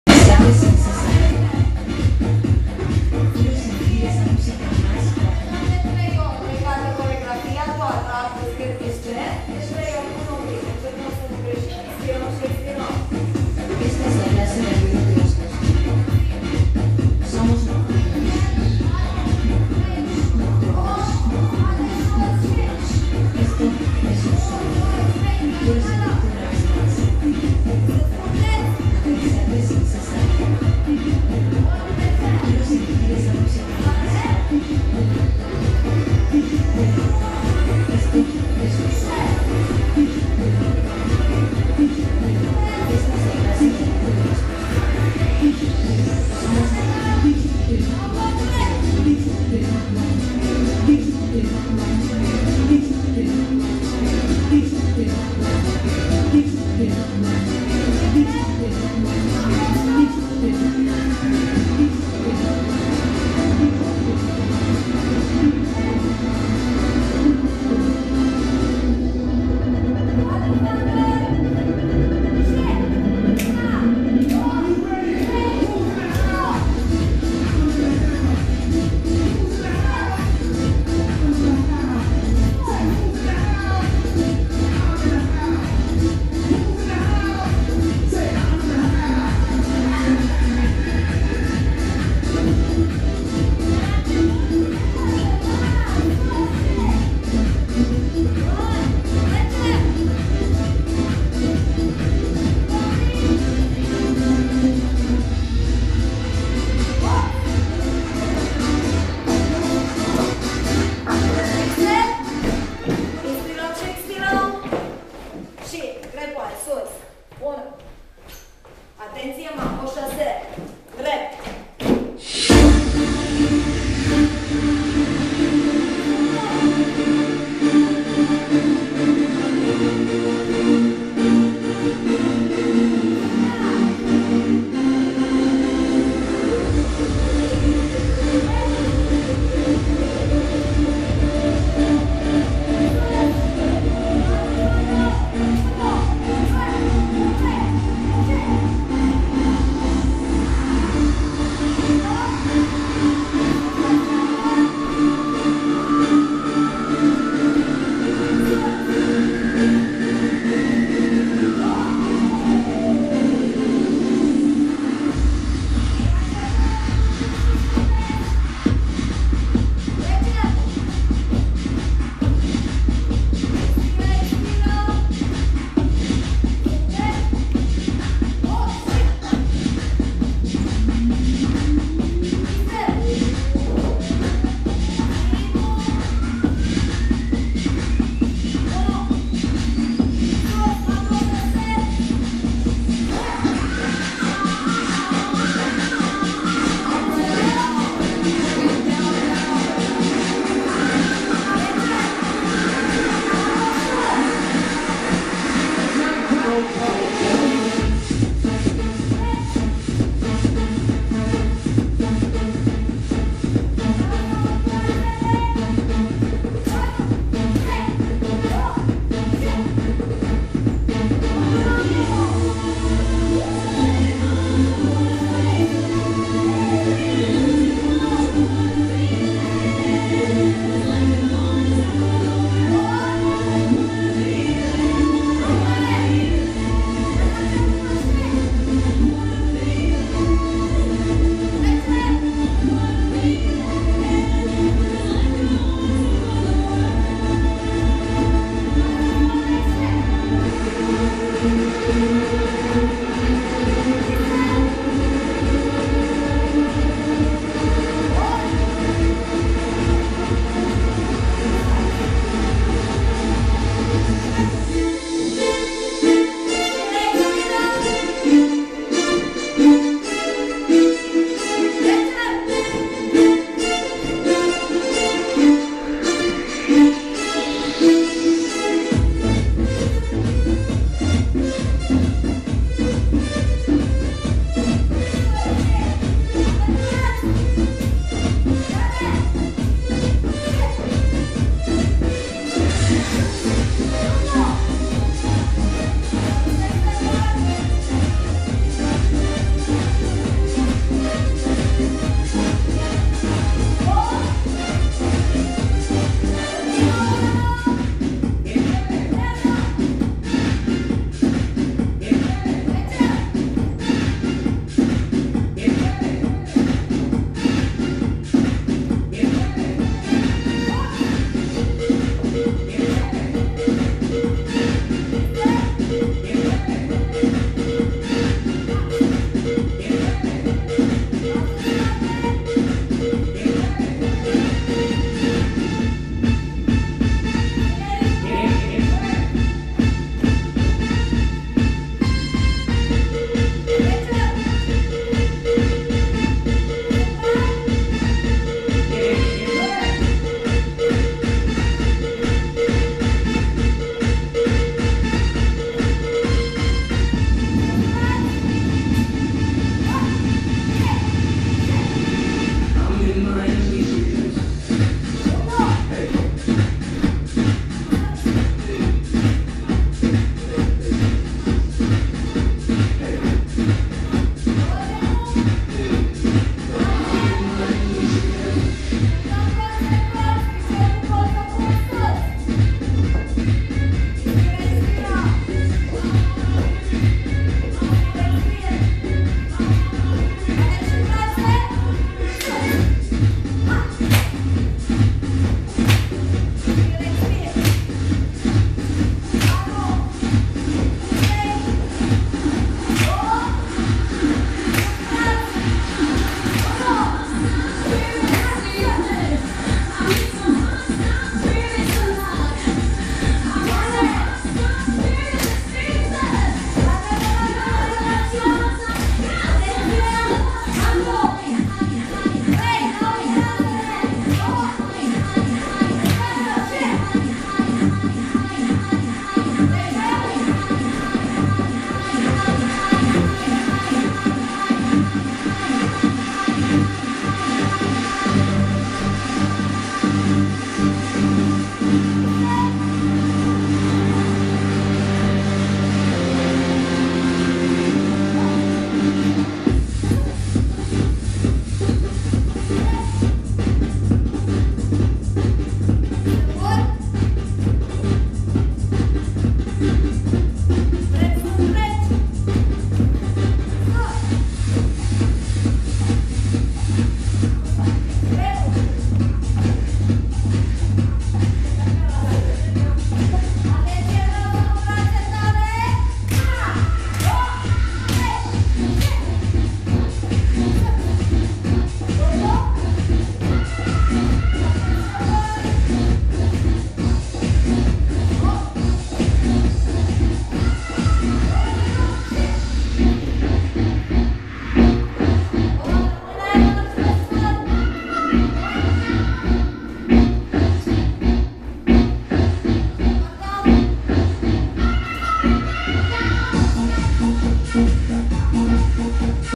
Curs Aerobic Cluj Napoca - Fitness Education
Curs Instructor Aerobic organizat de FItness Education School la Cluj Napoca
Master Trainer : Iulia Chiorean